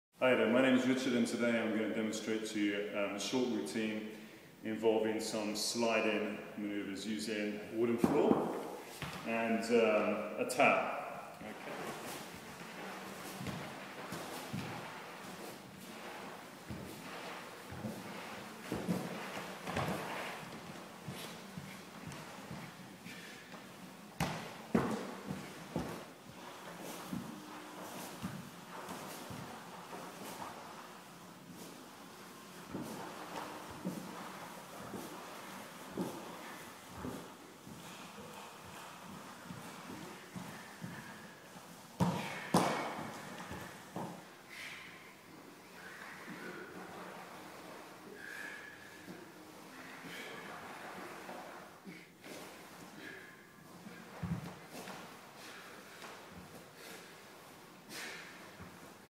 Short Sliding Exercise Routine
Great work for abs/torso, legs, arms, back in fact the whole body! Simply using a wooden floor & a small towel.